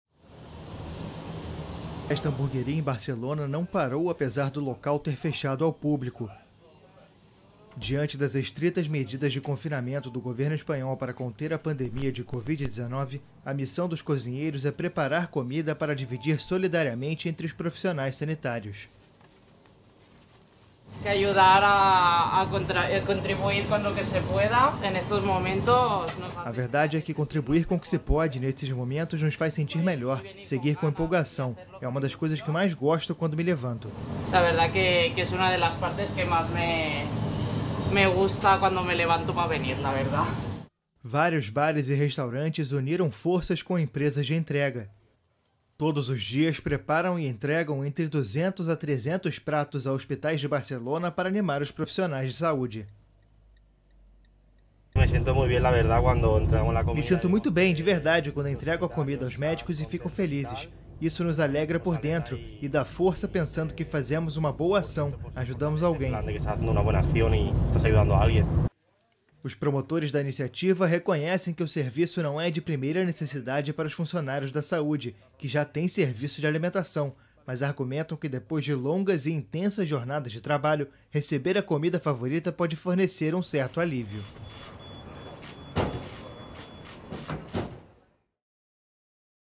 Restaurantes solidários na Espanha
Para ajudar profissionais da saúde, restaurantes funcionam até mesmo de portas fechadas. Todos os dias preparam e entregam entre 200 e 300 pratos a hospitais de Barcelona para animar os profissionais.

Reportagem:
AFP